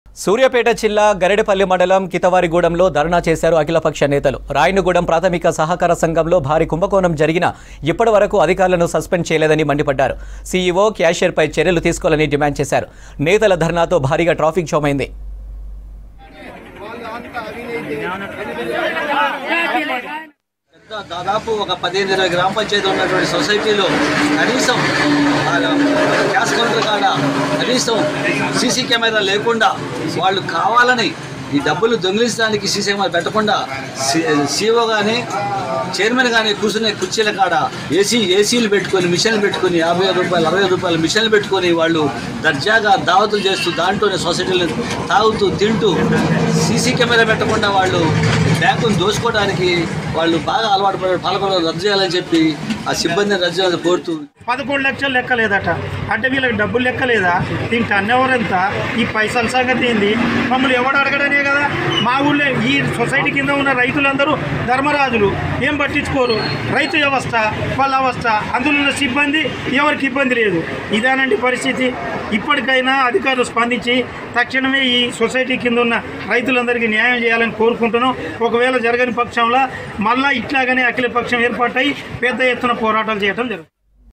All Party Leaders Protest At Keethavarigudem Over Corruption In Agriculture Office | Suryapet | V6
All Party Leaders Protest At Keethavarigudem Over Corruption In Agriculture Office | Suryapet | V6 News